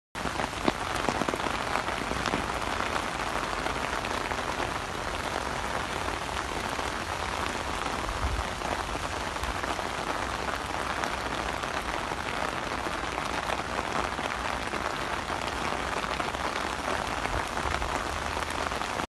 120 milioane spălați la prima ploaie
Strada Albișoara proaspăt reparată - inundată, chiar dacă ni s-a zis că problema va fi rezolvată. Sezonul ploilor e abia la început.

Cam așa arată lucrările de 120 milioane de lei, spălate la prima ploaie.

Dle Ceban, a mai fost un primar care a reparat înainte de alegeri strada Alecsandri, tot cu zeci de milioane de lei, tot fără scurgere pluvială, iar când plouă aceasta se transformă într-un râu care leaga Telecentru de Râșcani.

Înțeleg că sunt alegeri și șefu Dvs. vrea să câștige, dar nu faceți lucrări de mântuială din banii noștri și umblați cu minciuni. Fiecare leu cheltuit aiurea din banul public trebuie întors înapoi și oamenii care se fac vinoveți pentru lucrări necaltitative - trași la răspundere.